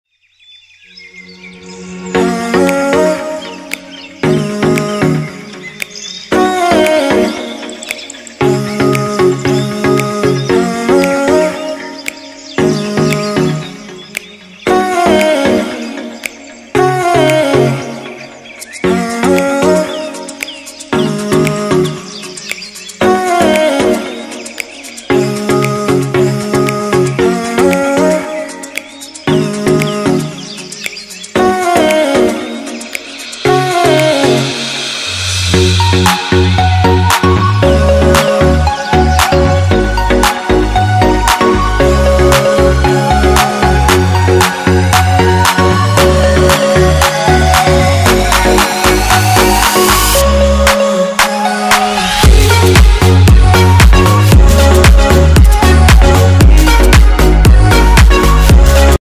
2024's Best Paper Plate Making Machine, Ultimate Buying Guide, Tirupathi
### Video Description:
Dive into the future of paper plate manufacturing with "Unveiling 2024's Elite Paper Plate Machines: Prices, Specs, & More!" 🚀 At AVR Paper Plate Making Machines, we're here to guide you through the latest advancements in paper plate technology. Discover the top machines of 2024, compare their prices, and specs, and uncover the features that set them apart. Whether you're a business owner looking to upgrade your equipment or a tech enthusiast curious about the latest industry trends, this video has everything you need. 🔔👍,